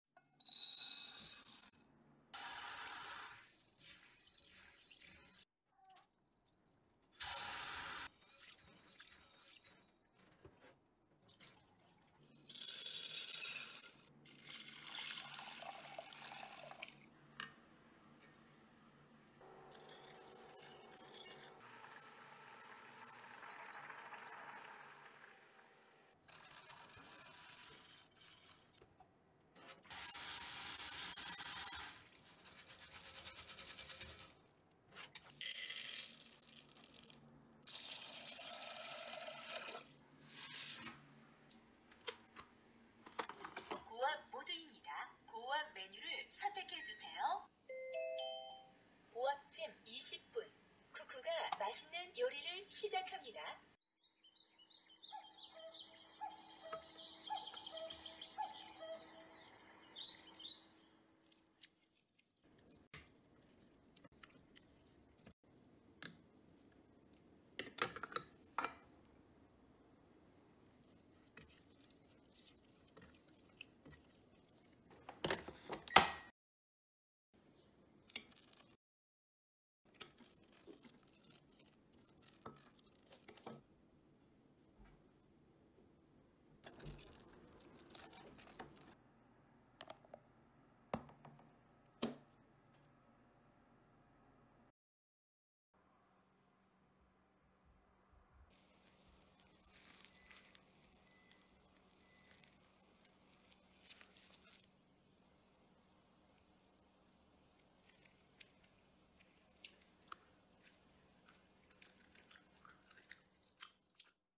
밥솥 저당 팥앙금🤎 믹서기 없이, 팥 불릴 필요 없이! How to make Red bean filling EASY RECIPE
You don't need to soak the red beans. It's a recipe for sweet red beans in an electric rice cooker
Refrigerate and use whenever necessary🤍

[Ingredients]
- Red beans, 200 g
- Sweetener (Erythritol) 70 g (7T)
- 3g of salt

In: recipig
*Unauthorized video theft, redistribution, and secondary editing uploads are prohibited.